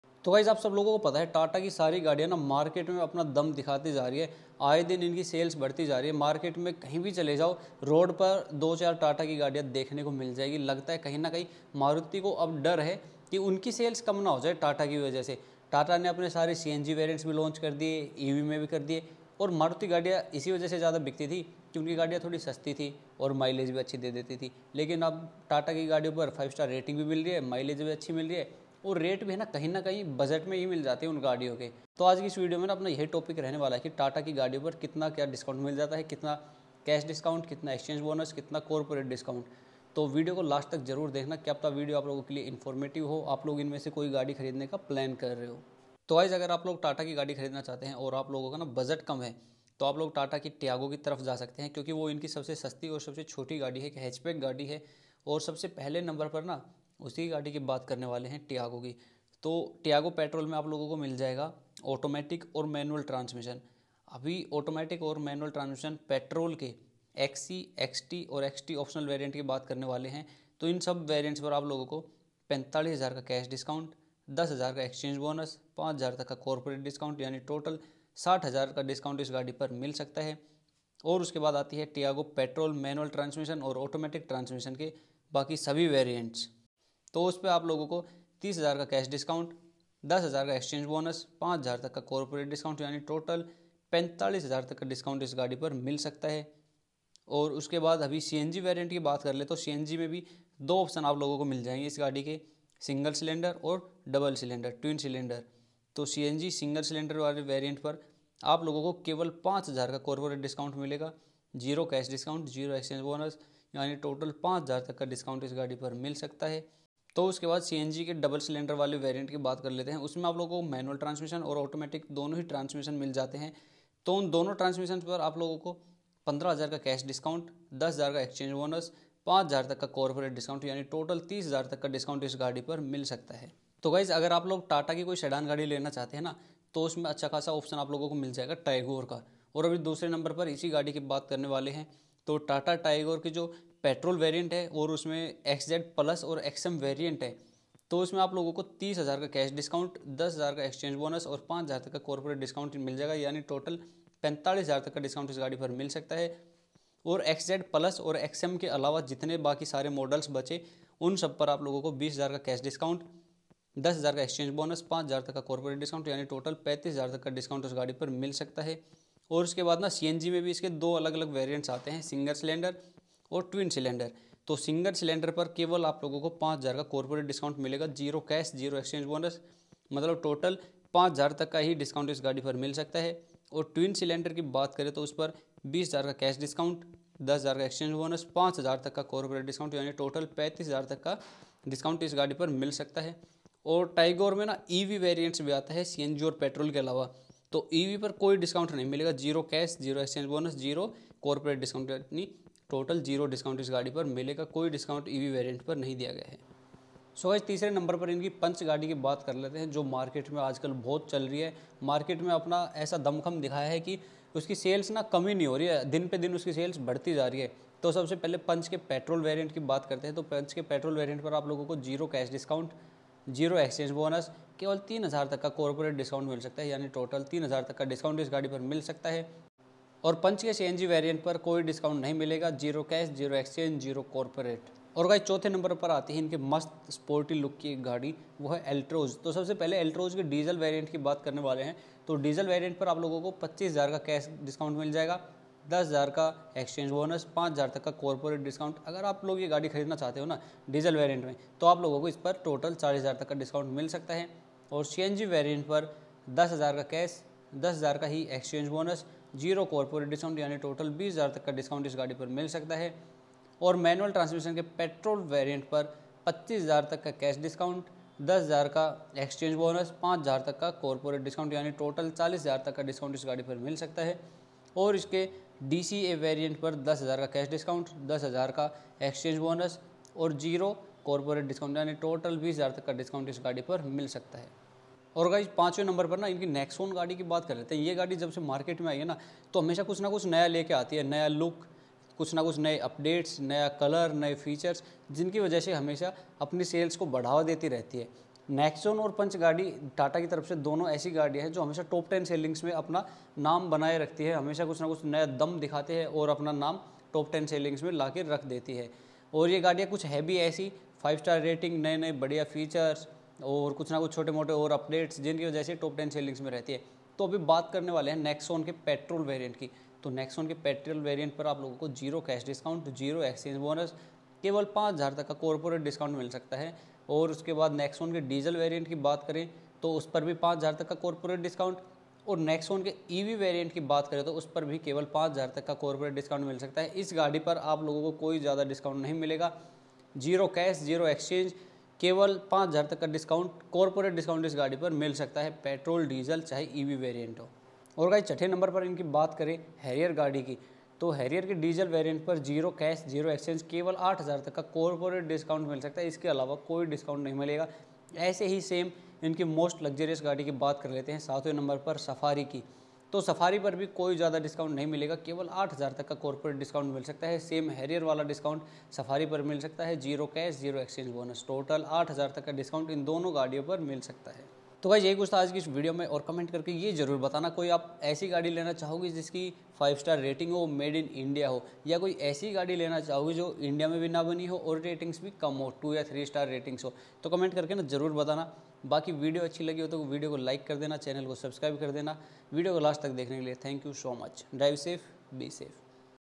Tata May 2024 Discount Offers : कौन सी गाड़ी लोगे अब 🔥 : DSD CARS
Hi Guys !
Aaj is video me May 2024 me Tata ki sabhi gaadiyo par milne wale discounts share kiye hai !